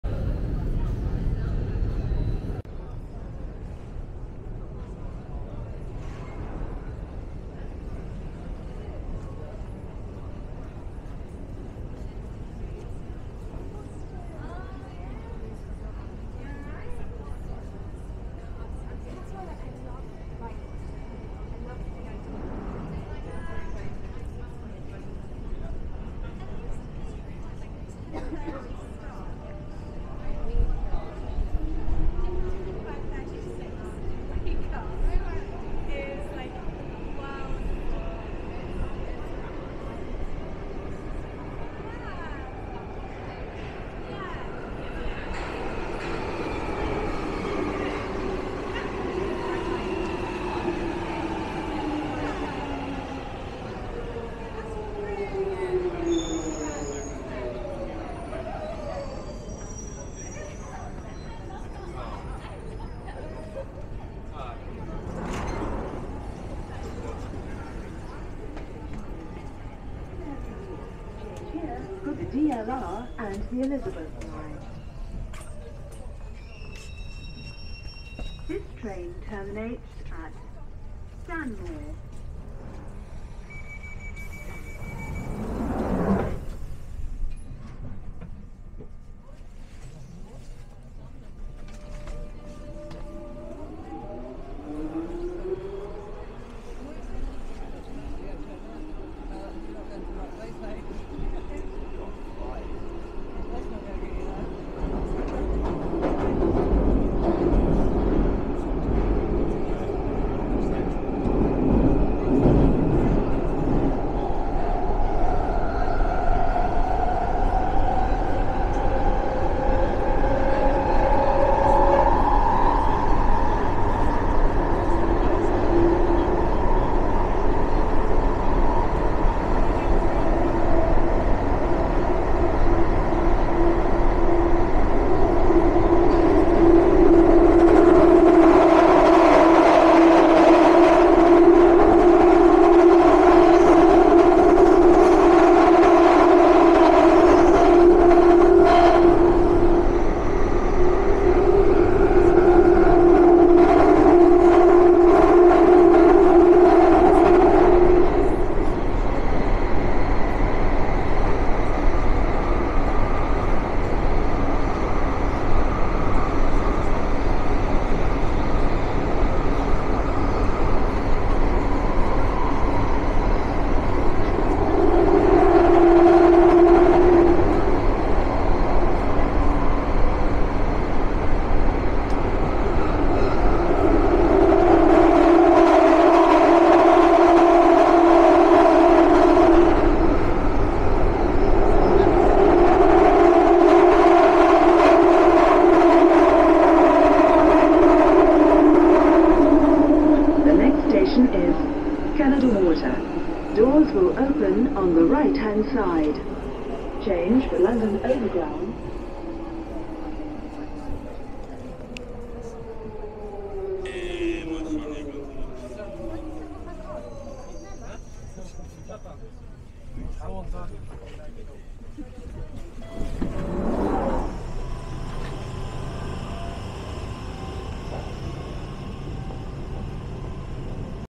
Canary Wharf to Canada Water Underground Station Tube Ride
London Canary Wharf to Canada Water Underground Station Tube Ride